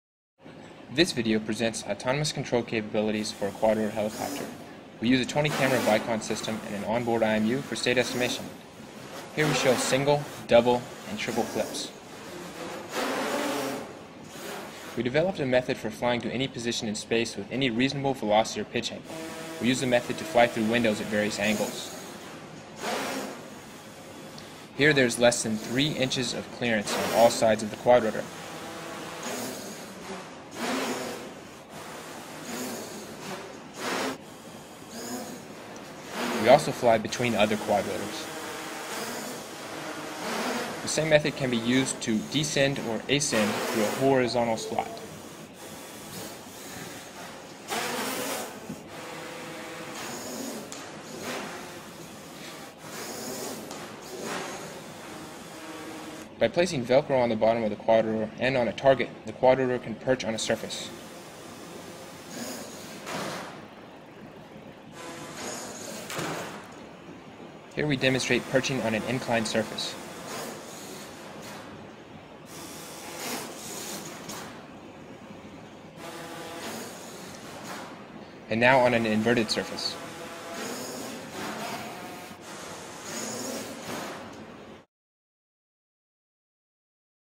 REAL HALF LIFE MANHACK -autonomous quadrotor helicopter robot flys through windows
Flying robot looks like it could be a little Terminator as it stops and starts in mid air.